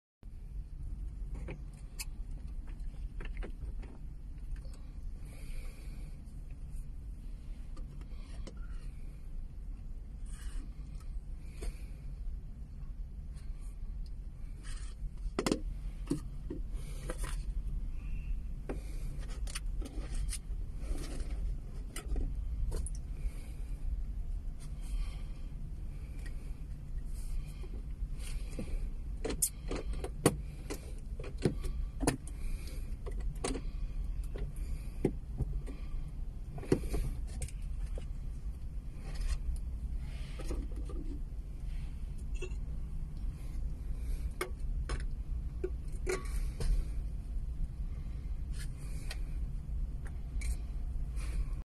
Honda civic 98 cup holder
This is a 3D printed cup holder for cigarette lighter port, made of ABS filament